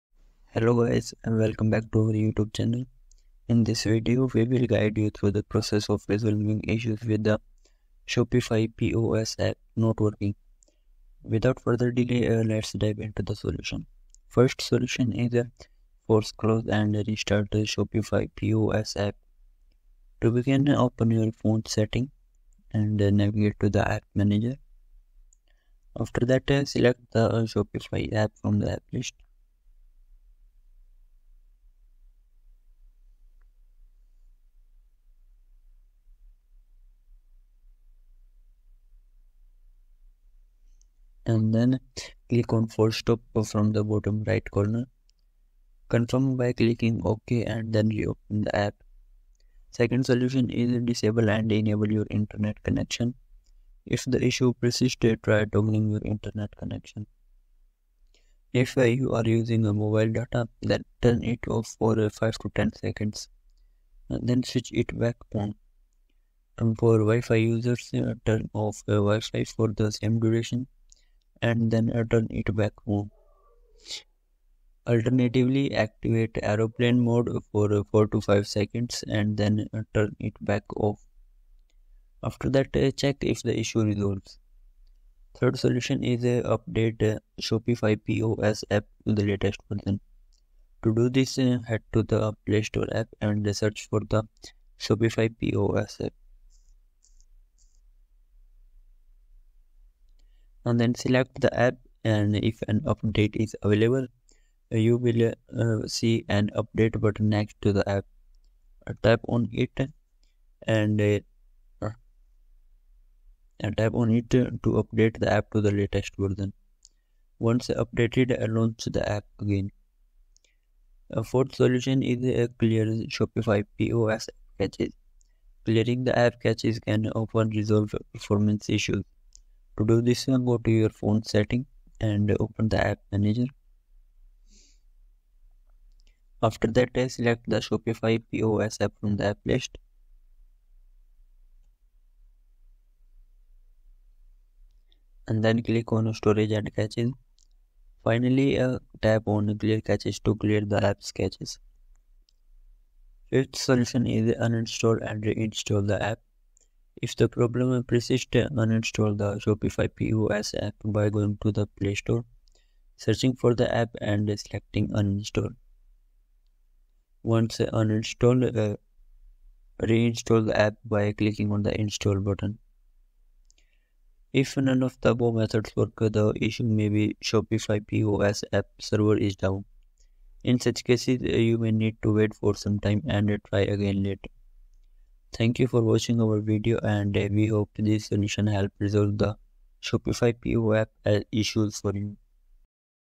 How To Fix Shopify POS App Not Working, Keep Crashing, Keep Stopping, Stuck On Loading Screen
In this tutorial, learn how to troubleshoot common issues with the Shopify POS app, such as it not working, crashing, stopping, or getting stuck on the loading screen. Follow step-by-step instructions to resolve these problems and get your app up and running smoothly again.

00.00  Introduction
00:14  Solution1 Force Close & Restart The Shopify POS App
00:49  Solution2 Disable And Enable Your Internet Connection
01:24  Solution3 Update The Shopify POS App To The Latest Version
02:00  Solution4 Clear Shopify POS App Cache
02:33  Solution5 Uninstall & Reinstall The Shopify POS App
02:53  Solution6 Wait For Sometime & Try Again